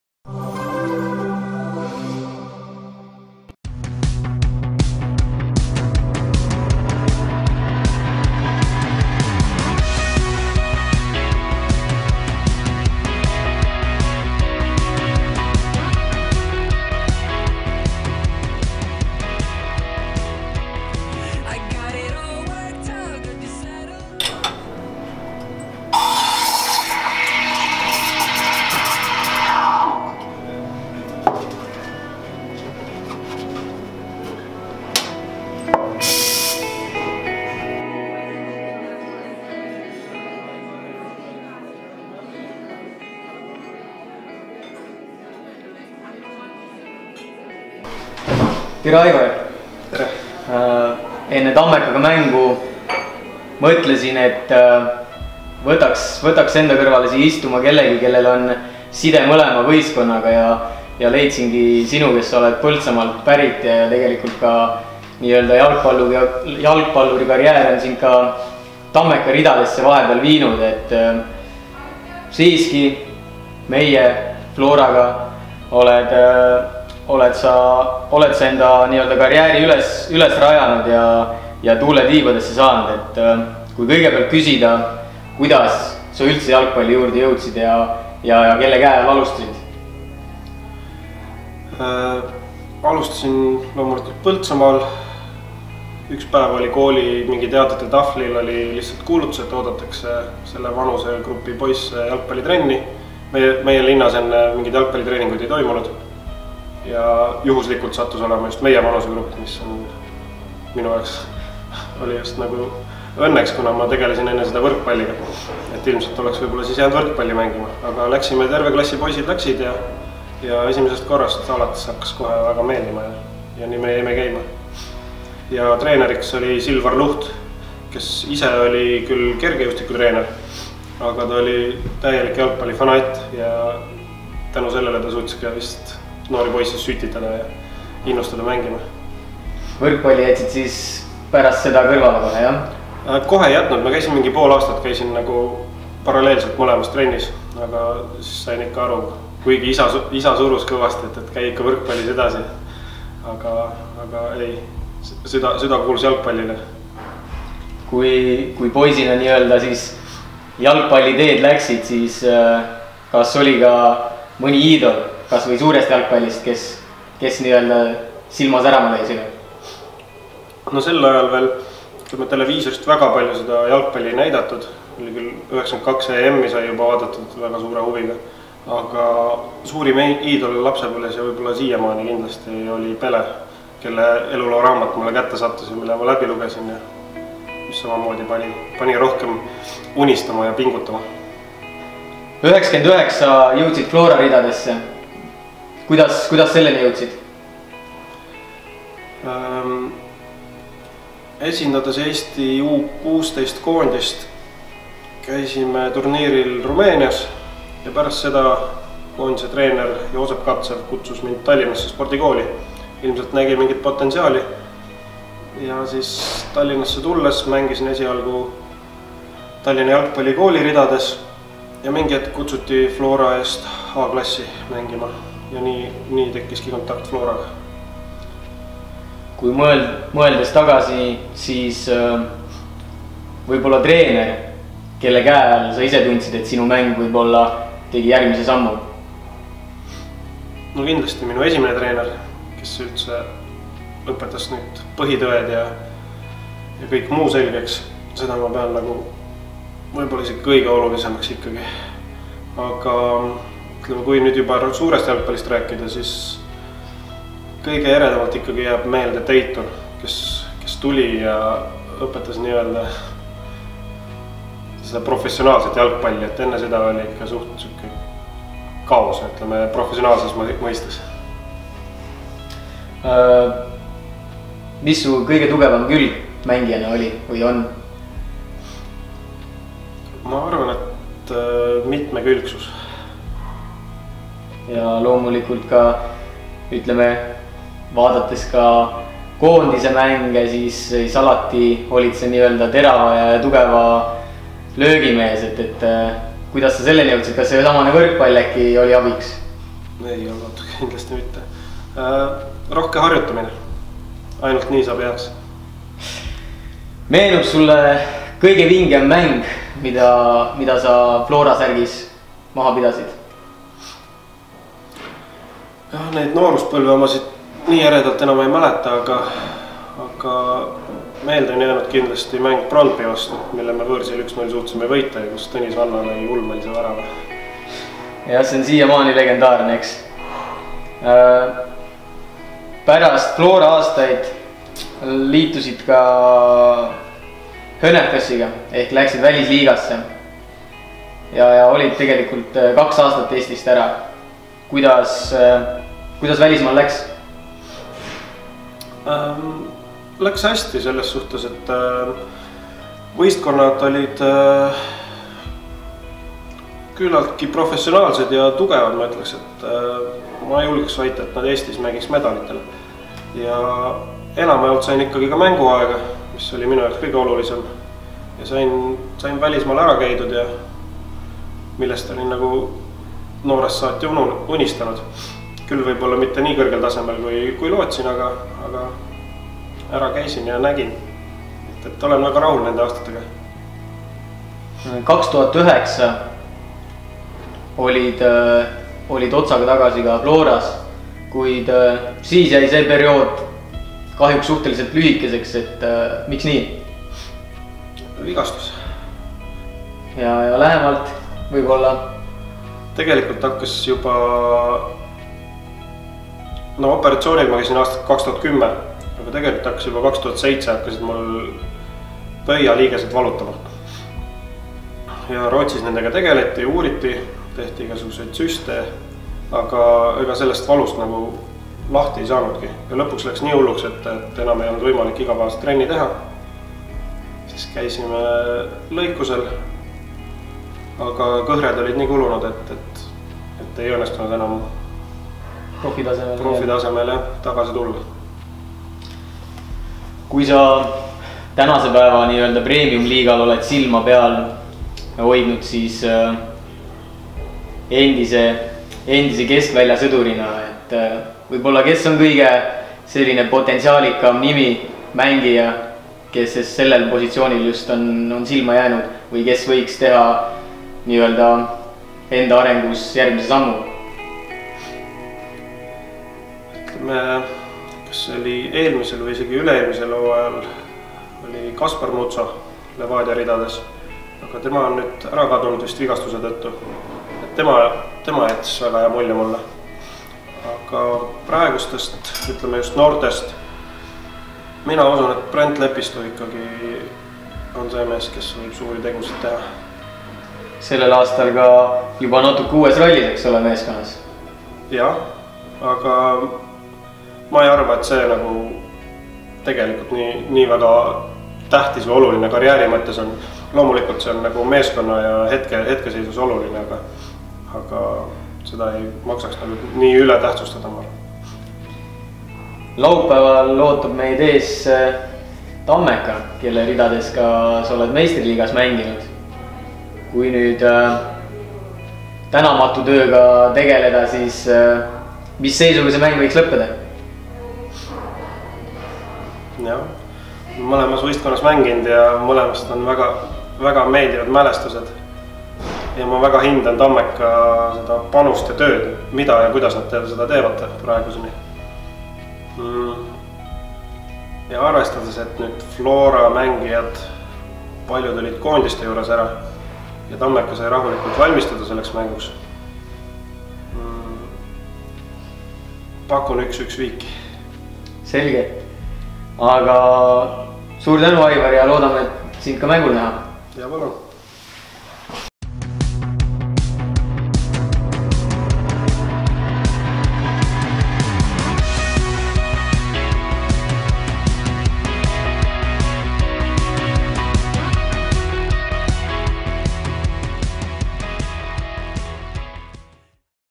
TV FLORA | Kohalik legend - Aivar Anniste!
Pärast koondise pausi ootab meid liigas põnev kohtumine, sest külla tuleb tabelinaaber Tartust - JK Tammeka. Enne mängu saime kokku meie Kohaliku Legendiga, kelle südames ka Tammekale koht leidub - Aivar Anniste!
Head vaatamist!